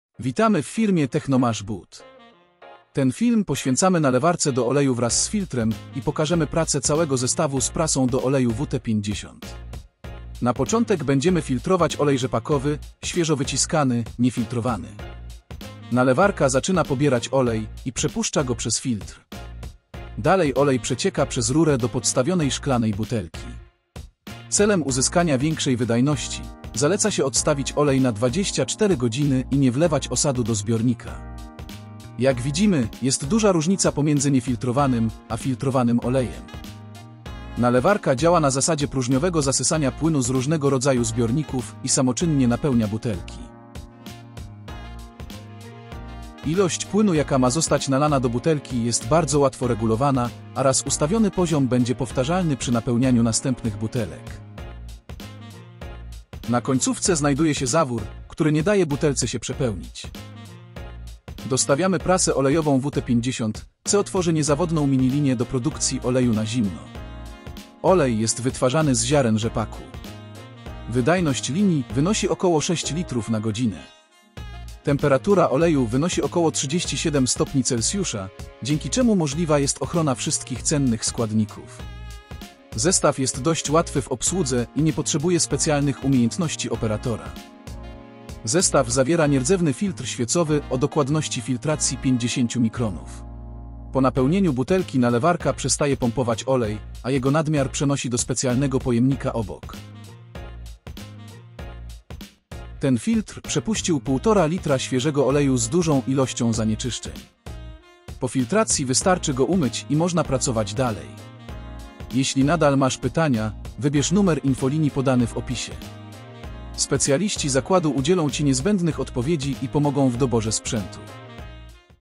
Mini linia do produkcji oleju na zimno
💥Wydajność - 6 l/h.*
*Wydajność może się różnić w zależności od typu surowca.

Prasa ślimakowa do tłoczenia oleju WT-50 firmy TechnoMaszBud jest przeznaczona do produkcji oleju metodą tłoczenia surowca na zimno. Ta maszyna pozwoli osiągnąć wydajność ok. 6 litrów na godzinę. Surowcami źródłowymi mogą być: nasiona lnu, nasiona sezamu, nasiona czarnego kminku, nasiona słonecznika, nasiona soi i wiele innych roślin oleistych.

Jeśli masz pytania – zadzwoń do nas. Fachowcy naszej firmy pomogą ci w wyborze sprzętu i udzielą wszystkich niezbędnych odpowiedzi.